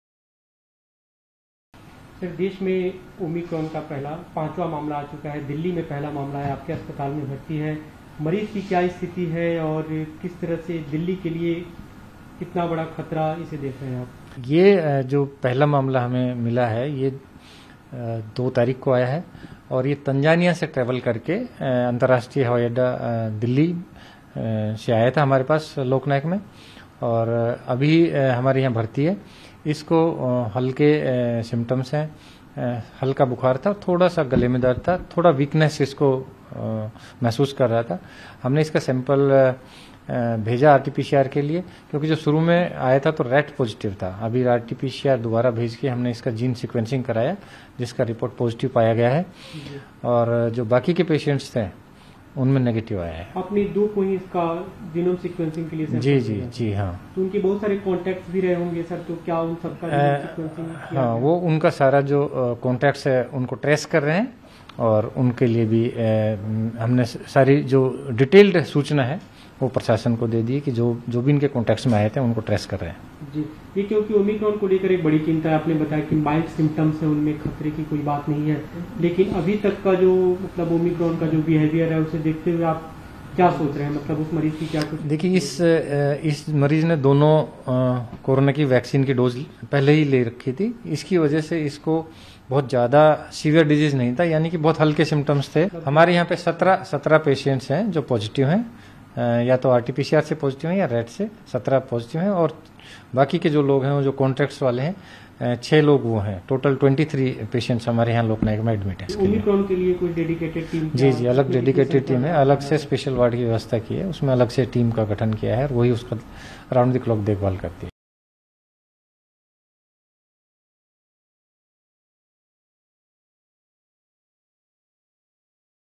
Omicron Cases India:दिल्ली के हॉस्पिटल में भर्ती Omicron के मरीज के बारे में डॉक्टर ने ये कहा
दिल्ली में कोरोना के नए वेरिएंट ओमिक्रोन (Omicron) का पहला केस सामने आया है। तंजानिया से लौटे व्यक्ति में ये नया वेरिएंट पाया गया है। दिल्ली के एलएनजेपी अस्पताल (LNJP Hospital) के डॉ सुरेश कुमार (Dr Suresh Kumar) ने रोगी के लक्षणों के बारे में जानकारी दी।